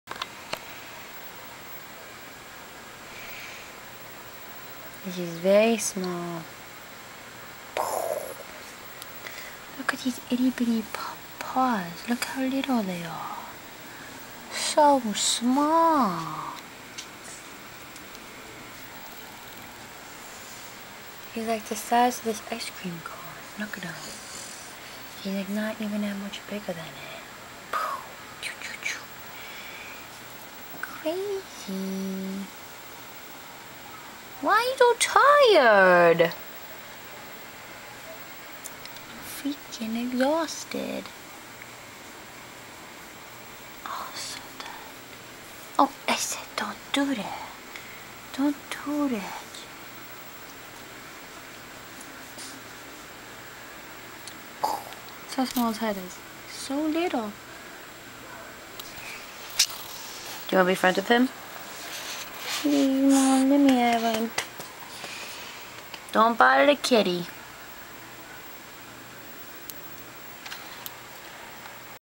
Stray Kitten
My boyfriend and I found this kitten walking home from doing our laundry. It was covered in pee and standing in garbage, so we took it home, gave it a bath, and fed it and now it sleeps and sleeps!